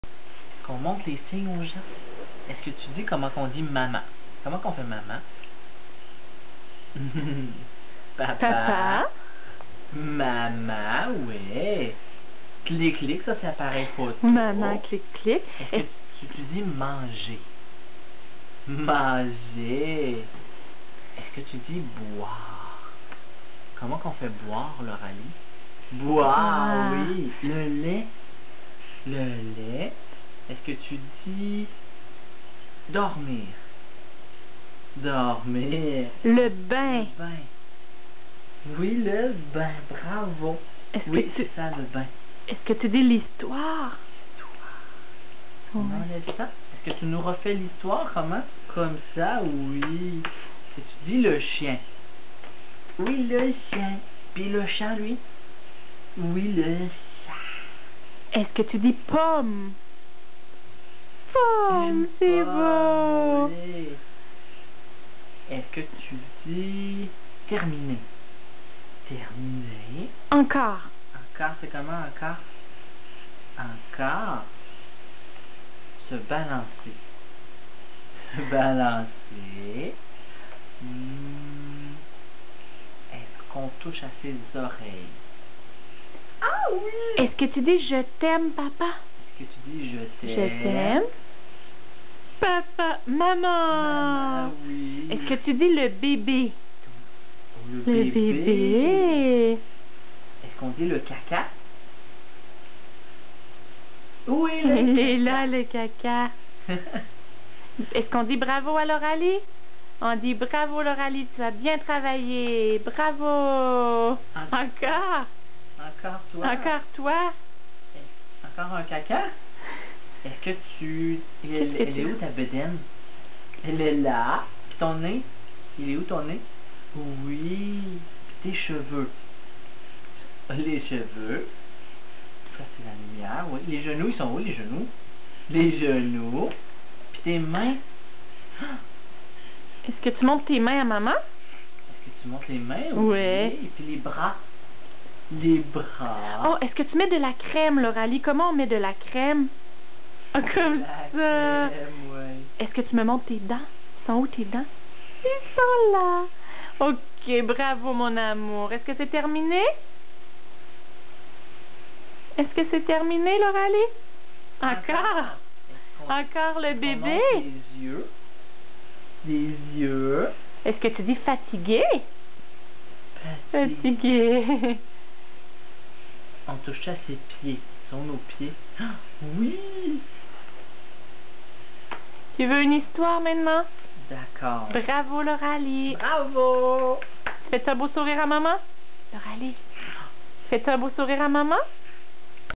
Lauralie qui parle le language signé bébé partie 1
Bienvenue dans le monde du langage de Lauralie, le langage des signes un peu adapté par notre petit trésor! Elle ne peut peut-être pas parler avec sa bouche, mais elle a très bien appris à utiliser ses petites mains pour communiquer et elle sait se faire comprendre! Évidemment certains signes sont à travailler, mais c'est comme un enfant qui apprend à parler, les mots ne sortent pas toujours correctement. Nous vous laissons découvrir son univers de la communication!!!